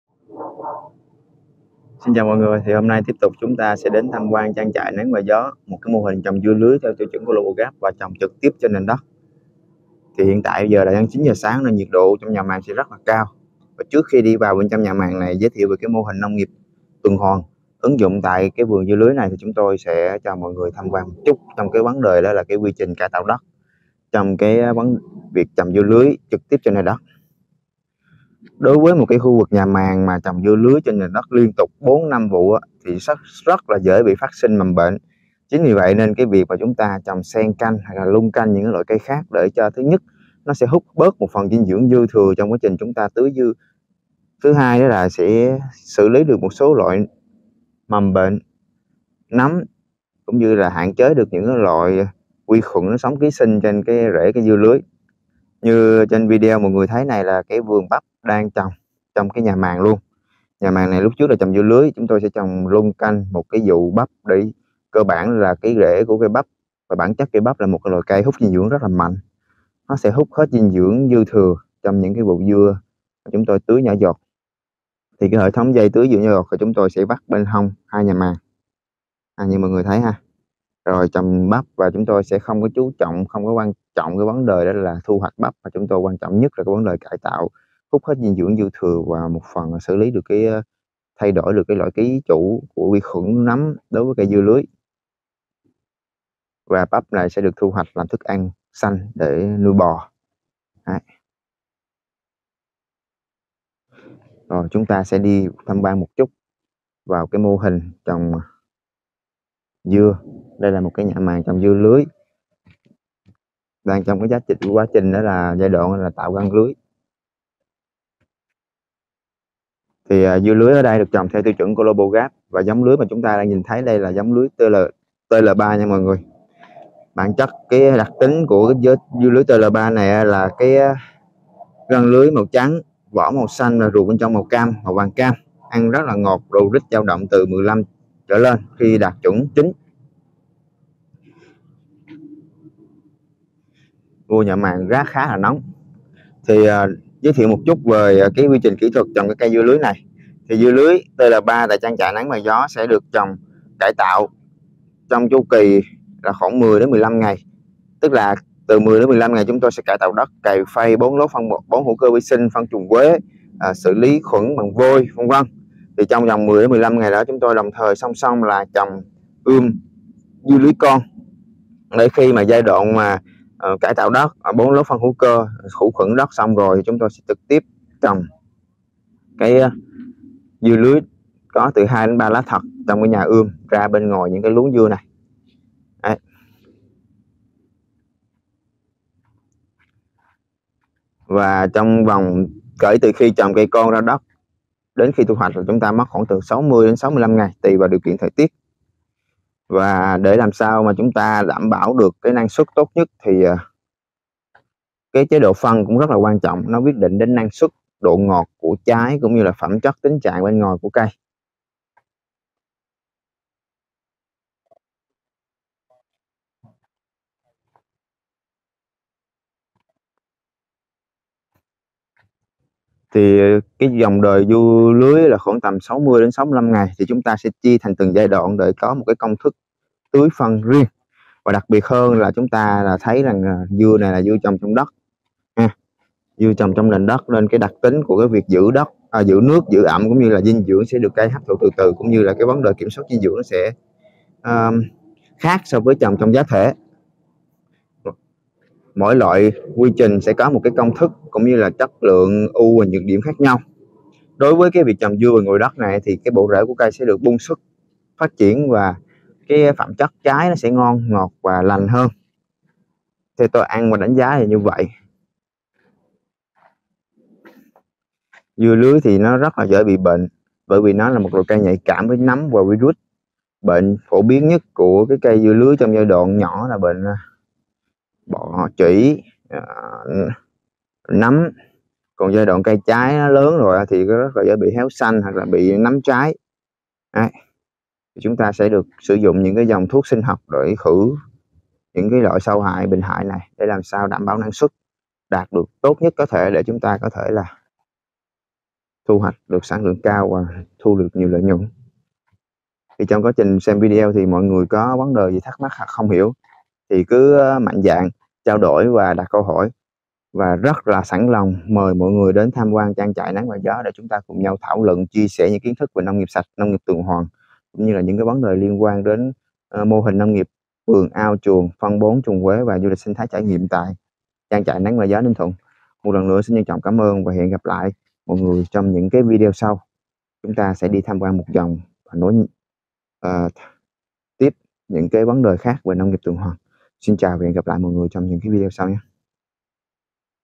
Tham quan trải nghiệm vườn dưa lưới trồng ngoài đất theo tiêu chuẩn Global GAP SW FARM năm 2022
Tham quan trải nghiệm vườn dưa lưới trồng ngoài đất theo tiêu chuẩn Global GAP tại trang trại Nắng và Gió Ninh Thuận năm 2022
DONATE tác giả Lê Minh Vương - TK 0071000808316 Ngân hàng Vietcombank, chi nhánh TpHCM.